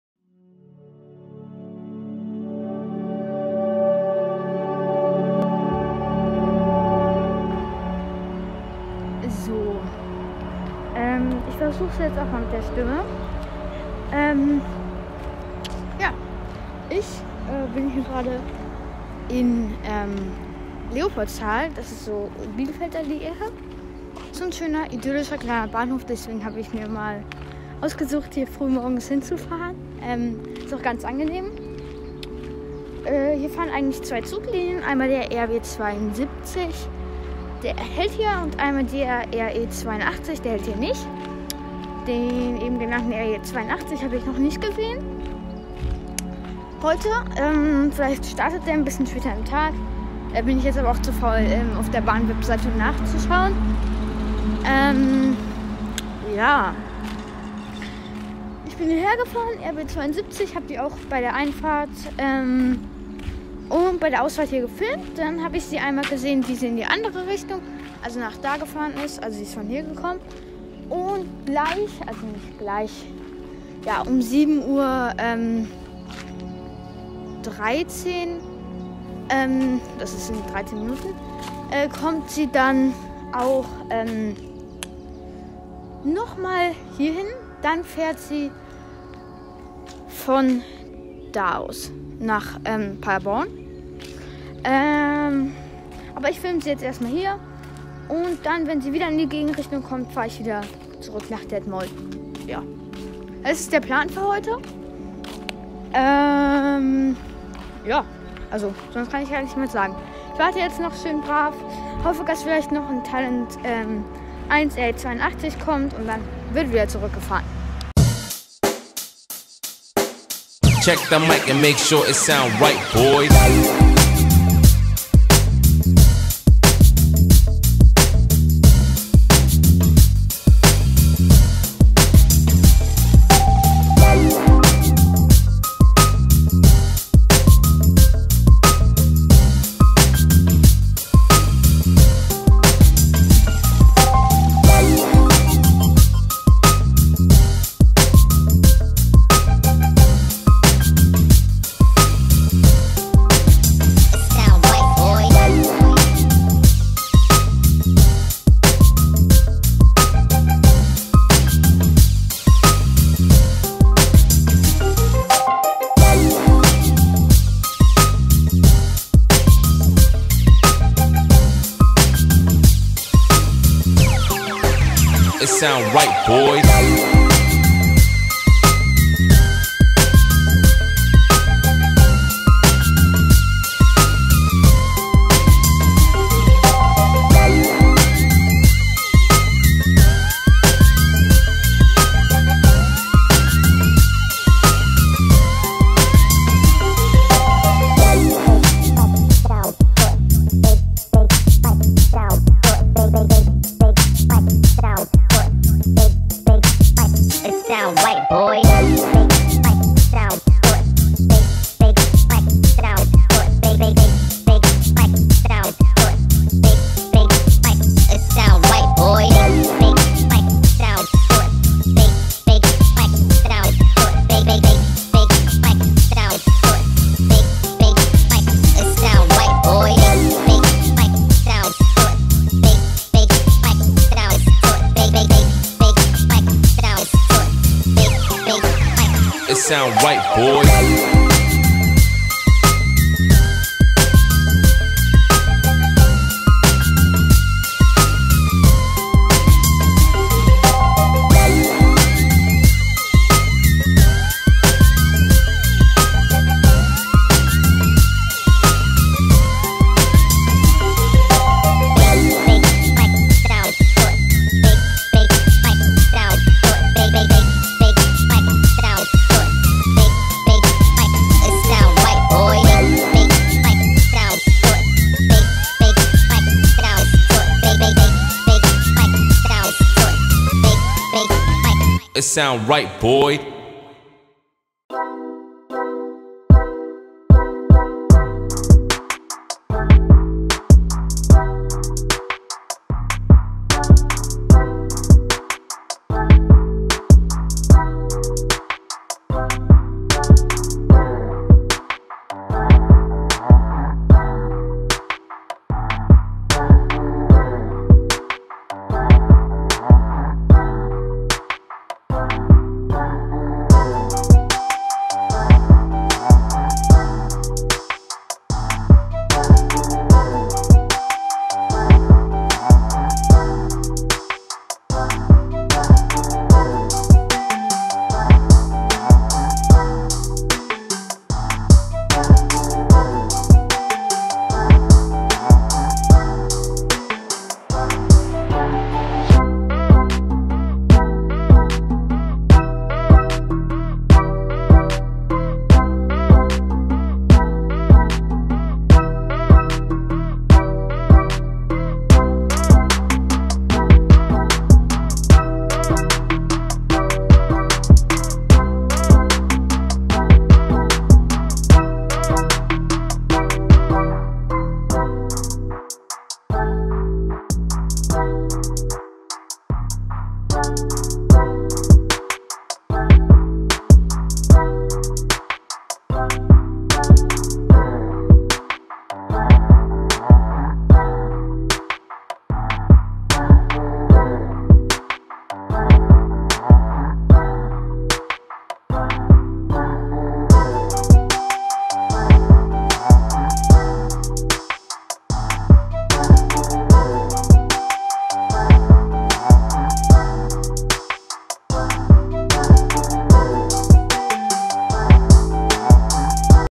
Trainspotting in Leopoldstal
Heute Morgen bin ich nach Leopoldstal gefahren und habe ein bisschen Züge gefilmt, hier das Ergebnis